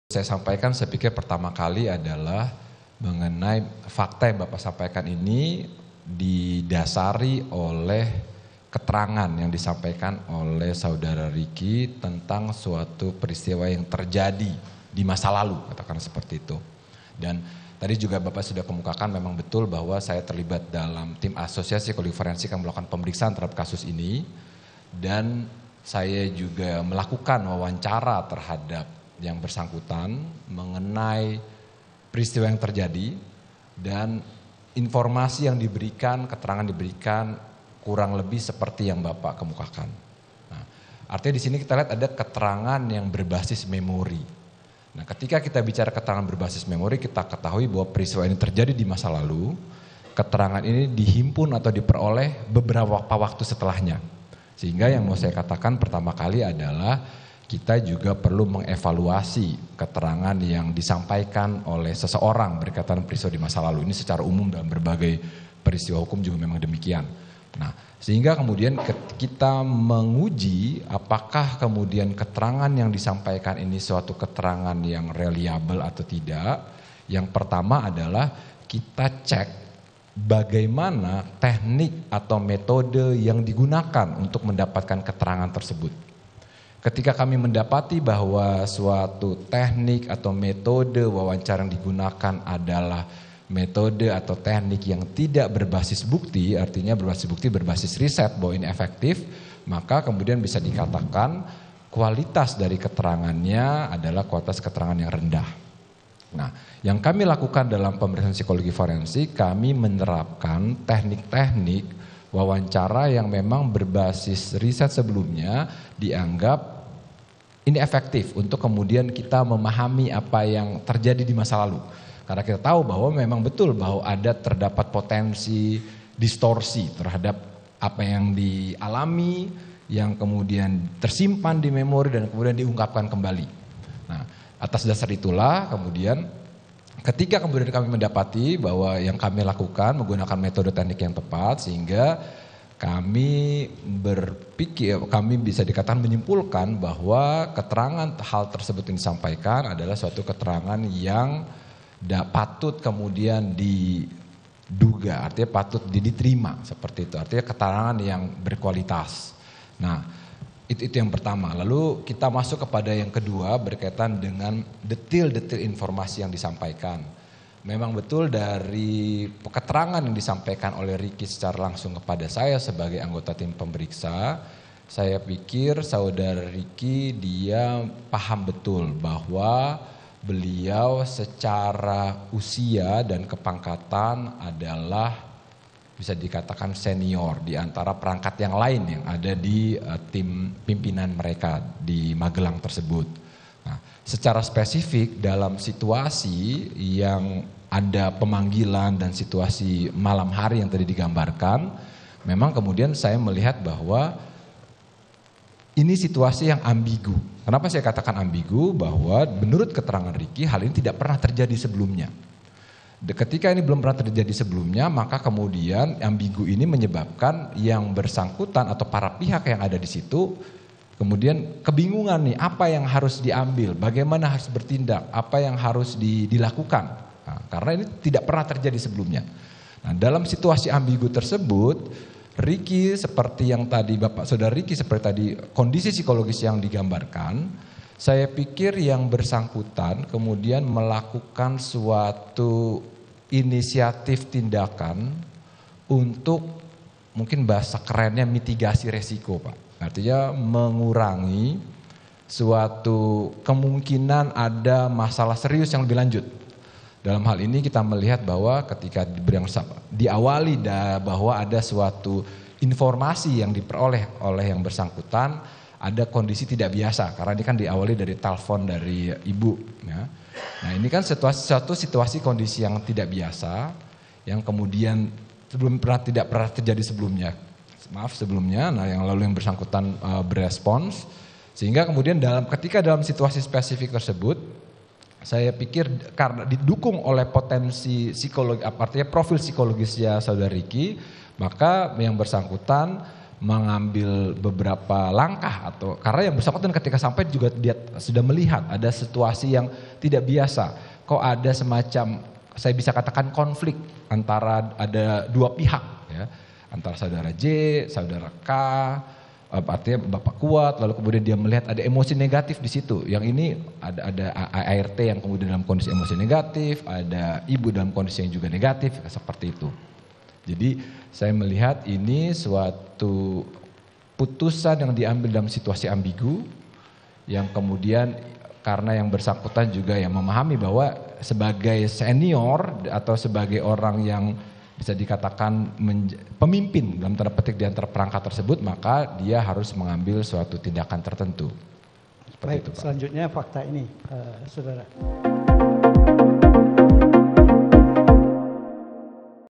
Kata Ahli Psikologi Forensik Soal Perilaku Ricky Rizal saat di Magelang
JAKARTA, KOMPAS.TV - Ahli Psikologi Forensik Nathanael Johanes tanggapi pertanyaan Penasihat hukum Ricky Rizal terkait perilaku Ricky Rizal saat di Magelang.

Nathanael menjelaskan bahwa berdasarkan keterangan yang dijelaskan oleh Ricky bahwa situasi di Magelang merupakan Ambigu karena tidak pernah terjadi sebelumnya.

Baca Juga [FULL] Kata Kuasa Hukum Ricky Rizal Usai Dengar Keterangan Ahli Psikologi Forensik di

Nathanael menilai Ricky melihat situasi yang tidak biasa sehingga melakukan tindakan seperti mitigasi risiko.

Hal ini disampaikan Ahli saat berikan keterangan sebagai ahli meringankan yang didatangkan penasihat hukum Ricky Rizal di sidang lanjutan pembunuhan berencana Yosua di Sidang Lanjutan pada Senin (2/1/2023)